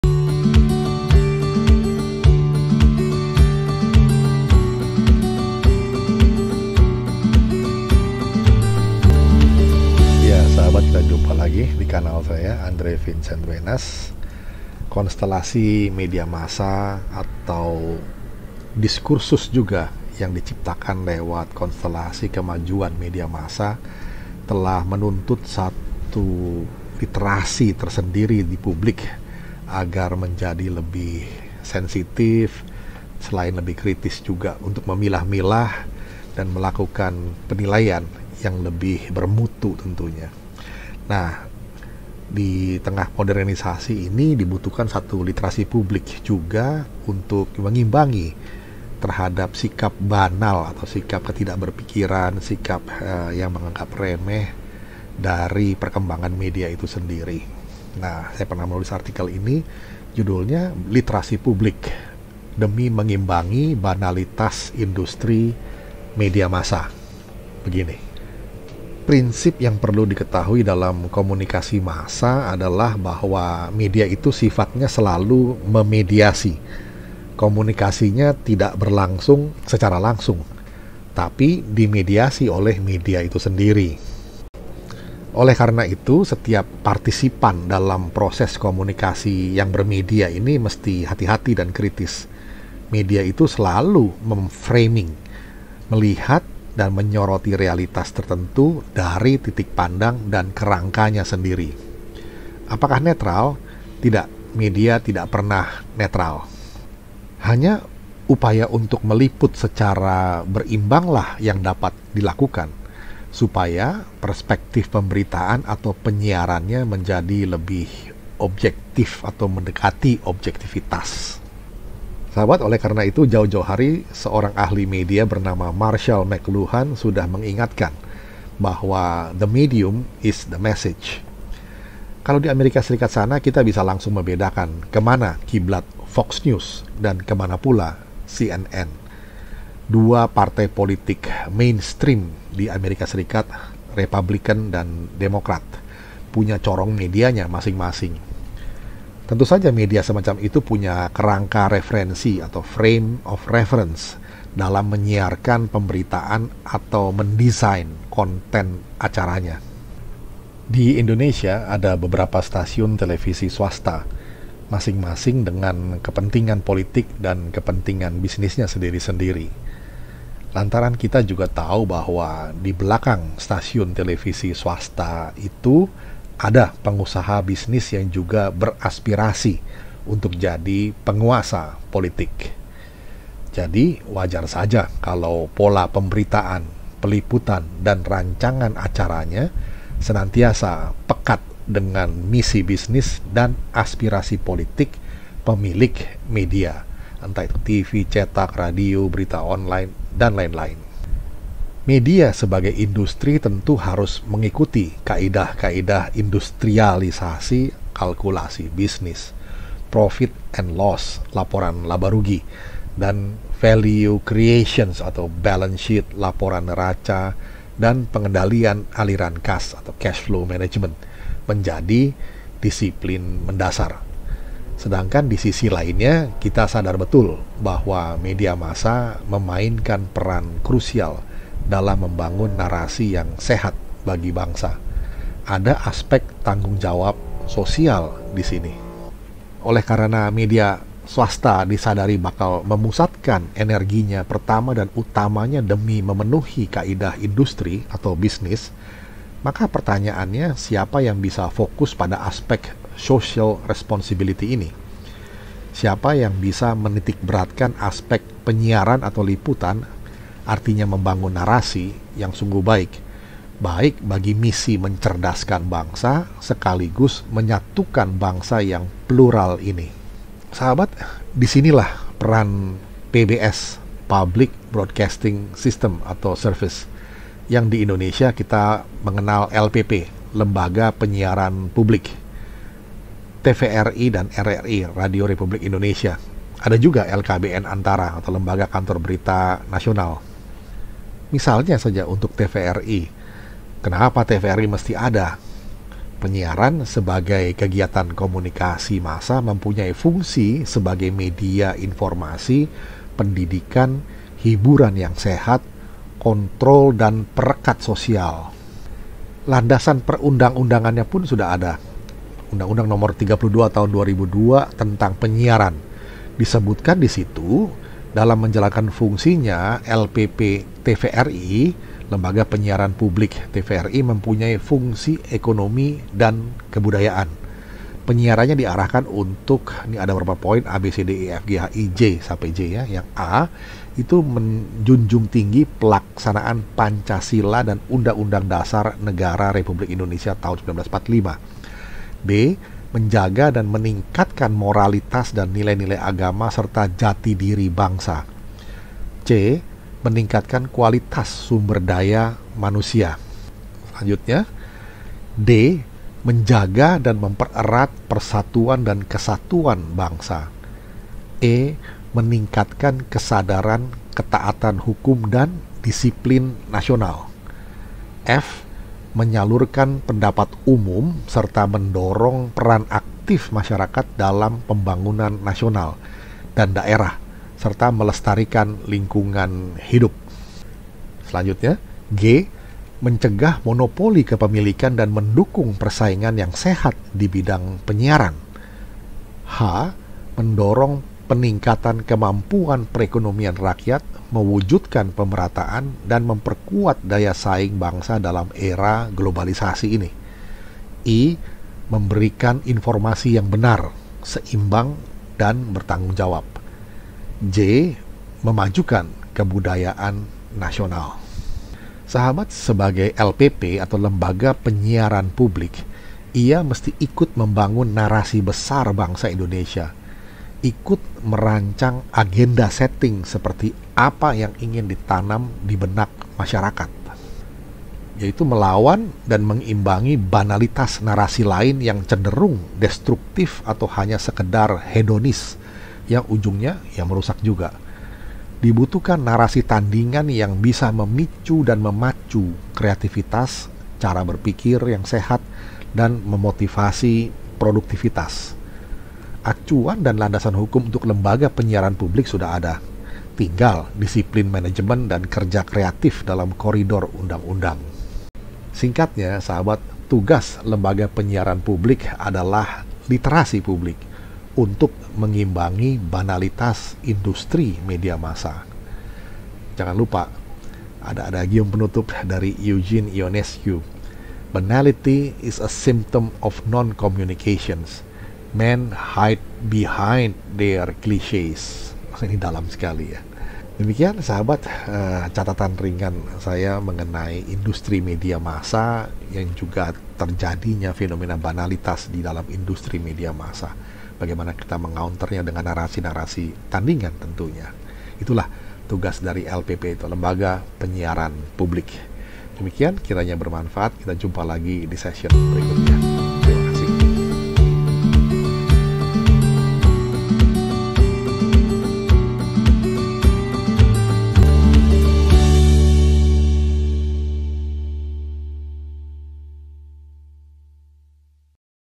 LITERASI PUBLIK: MENGIMBANGI BANALITAS INDUSTRI MEDIA MASSA
LPP (lembaga penyiaran publik) mesti ikut membangun narasi besar bangsa Indonesia. Ikut merancang agenda-setting seperti apa yang ingin ditanam dibenak masyarakat. Melawan dan mengimbangi banalitas narasi lain yang cenderung destruktif atau hanya sekedar hedonis yang ujungnya merusak juga.

Link artikel:

Andre Vincent Wenas, DRS,MM,MBA.